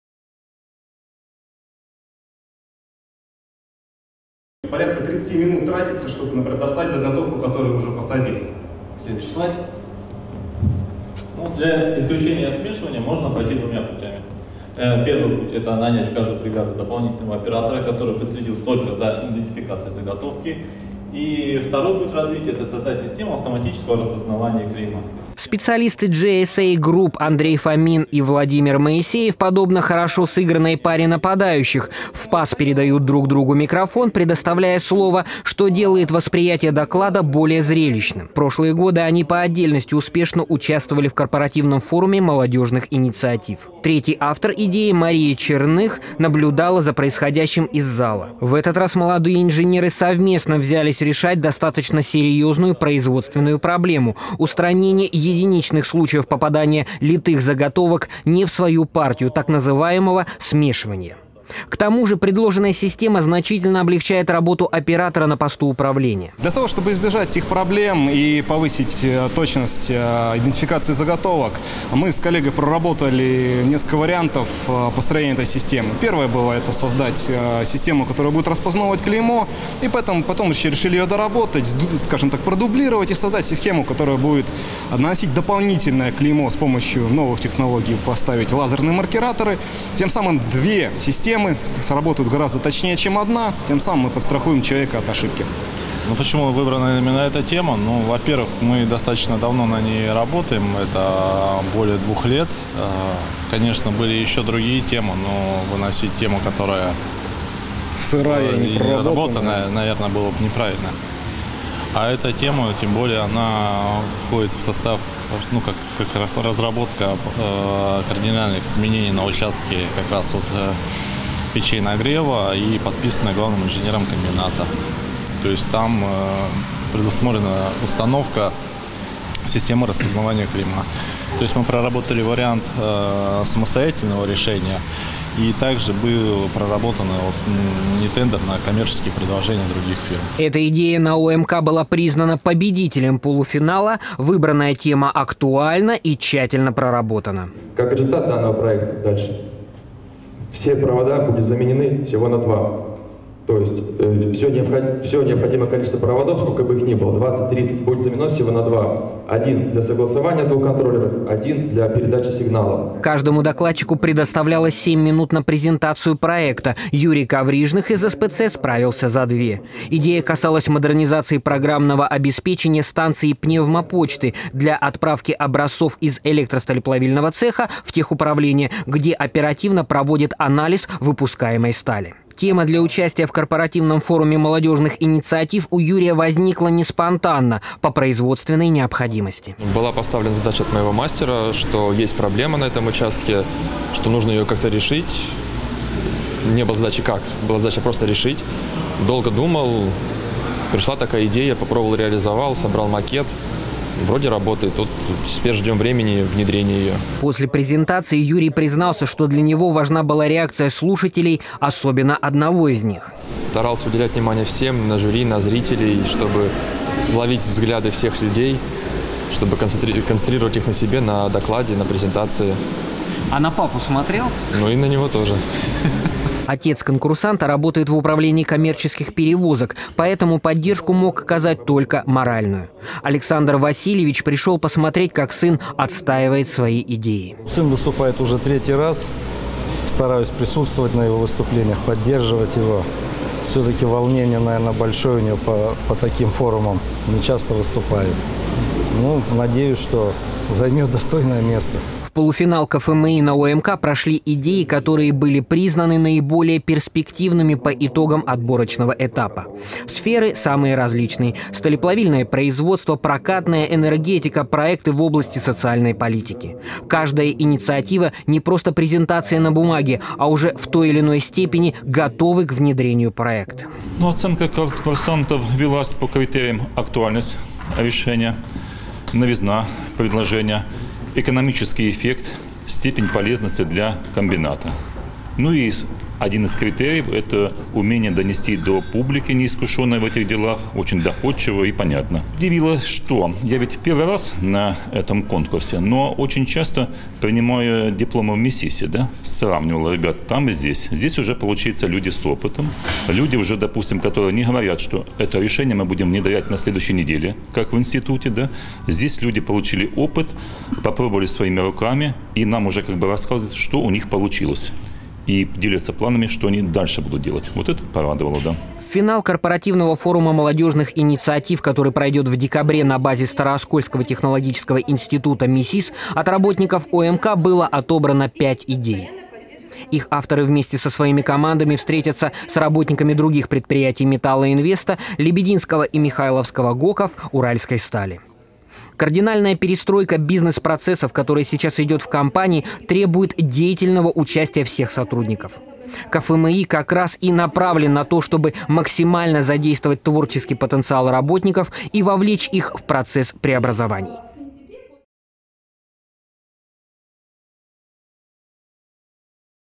На ОЭМК определились финалисты III Корпоративного форума молодежных инициатив
В полуфинальном этапе ОЭМК на суд компетентного жюри было представлено шесть работ специалистов комбината. Их проекты касались как повышения эффективности производства, так и социальных программ. Форум проводится в компании Металлоинвест уже третий год подряд и позволяет проявить себя наиболее инициативным молодым сотрудникам, дает им дополнительные возможности для профессионального роста и самореализации.